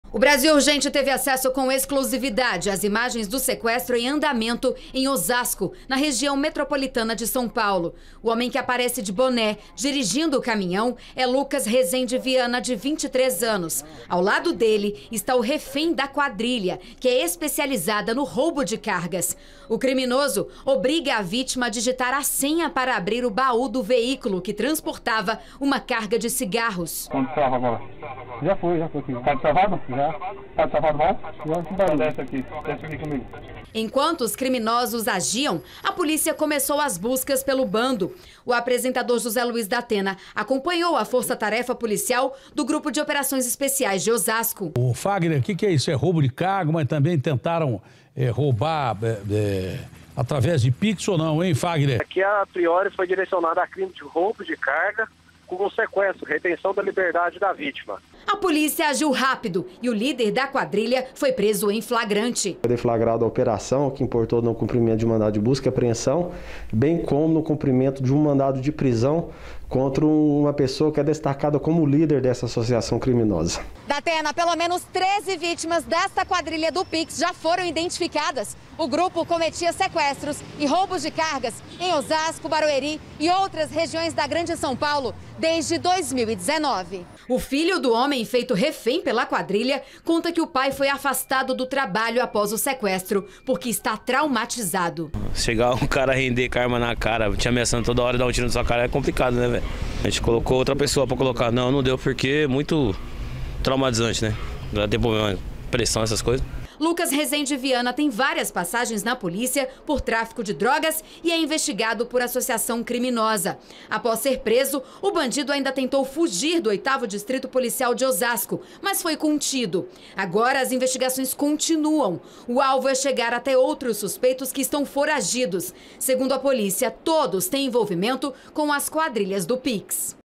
PIX: MOTORISTA REFÉM
Bandidos deixam refém traumatizado com a violência